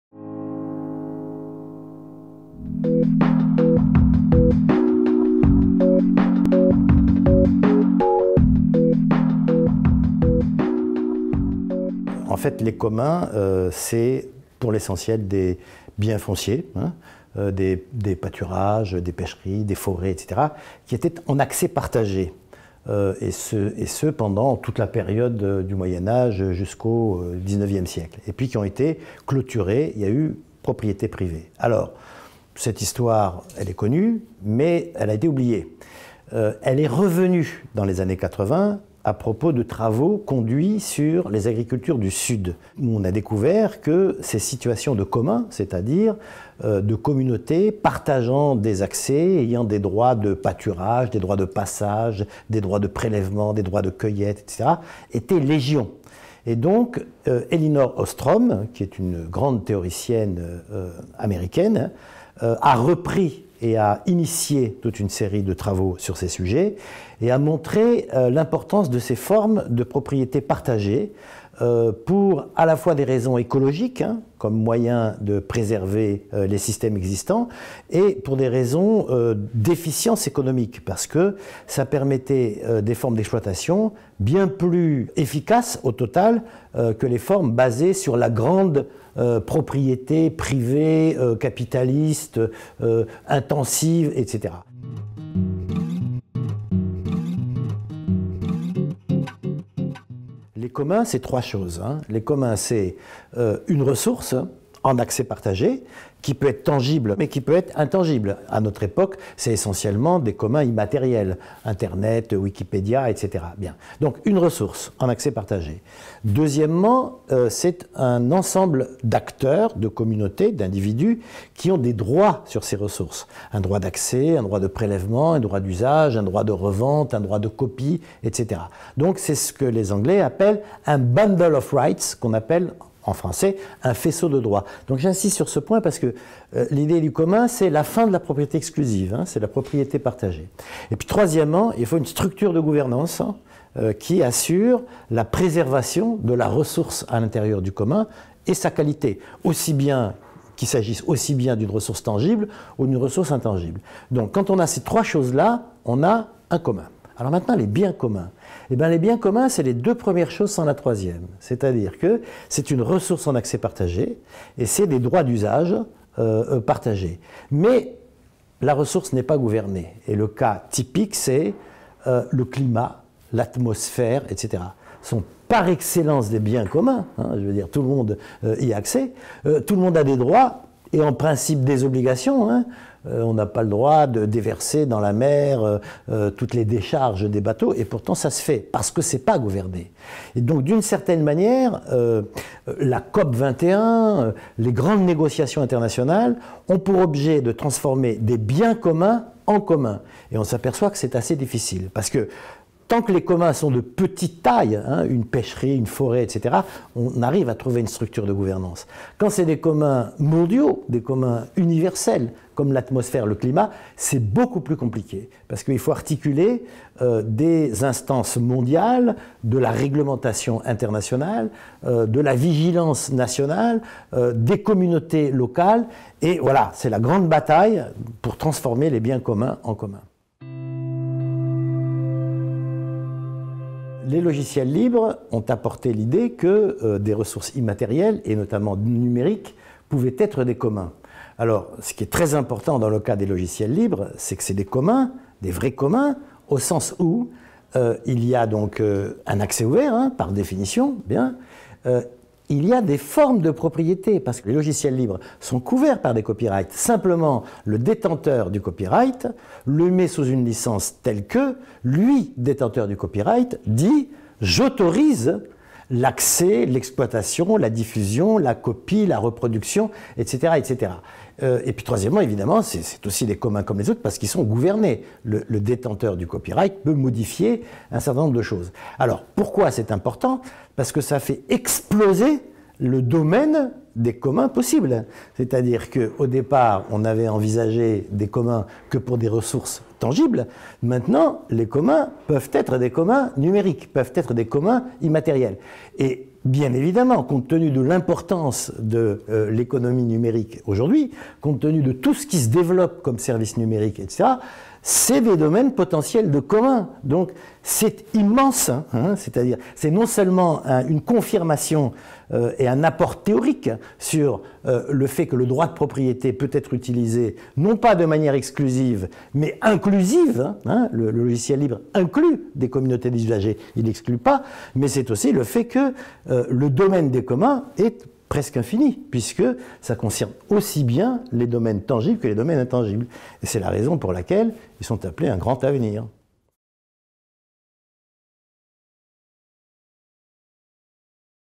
Les Communs : Interview de Benjamin Coriat
Entretien avec Benjamin Coriat, économiste et spécialiste de l'innovation et de la propriété intellectuelle qui nous éclaire sur la notion des "Communs".